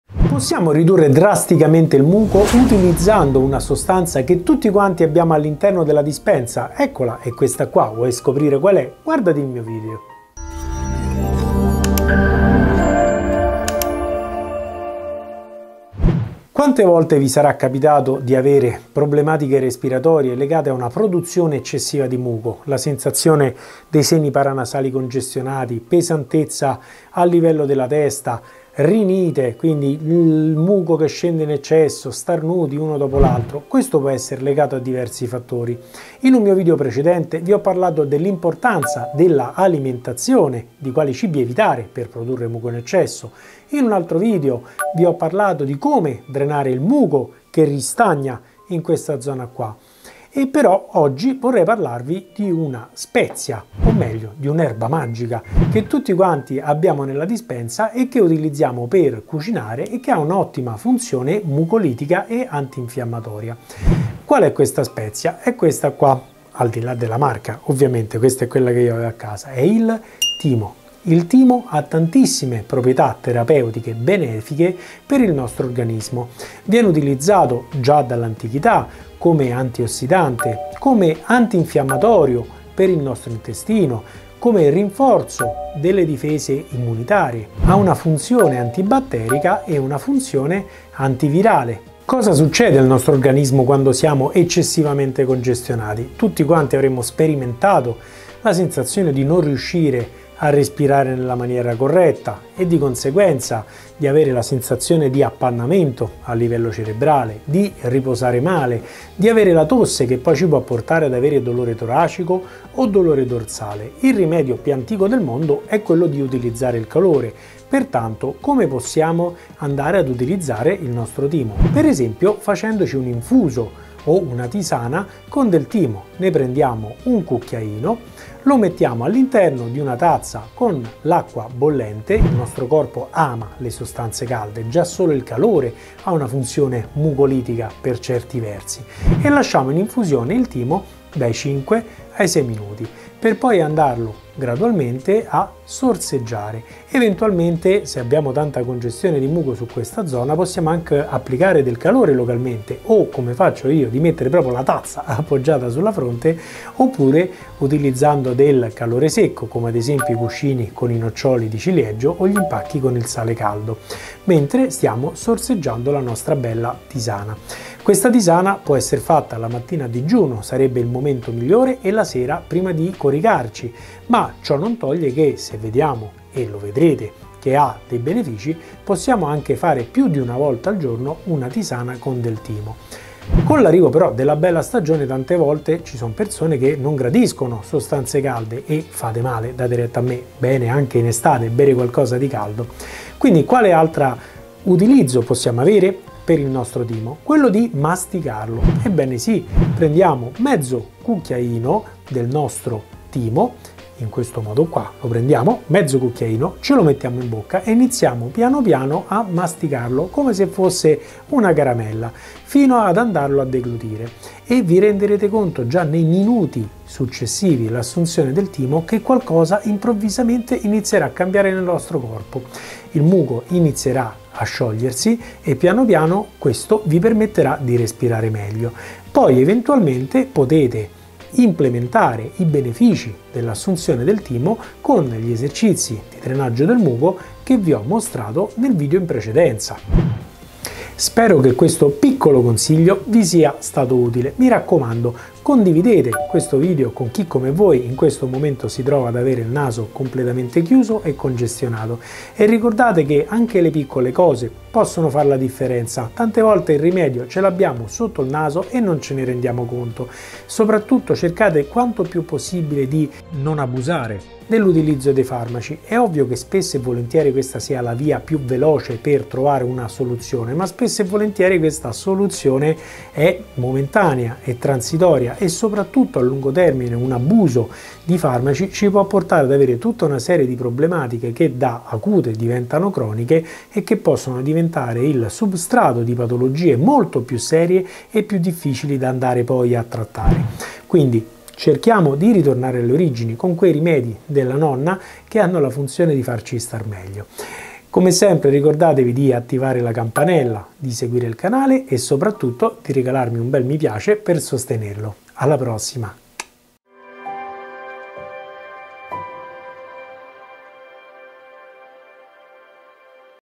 Questa spezia che hai in casa scioglierà subito il tuo MUCO in ECCESSO
Lo sapevi che per sciogliere il muco ti basta usare una spezia che hai nella credenza?
Puoi farci una tisana oppure masticarla e il suo effetto mucolitico ed antinfiammatorio ti libererà in un attimo.
Prova e fammi sapere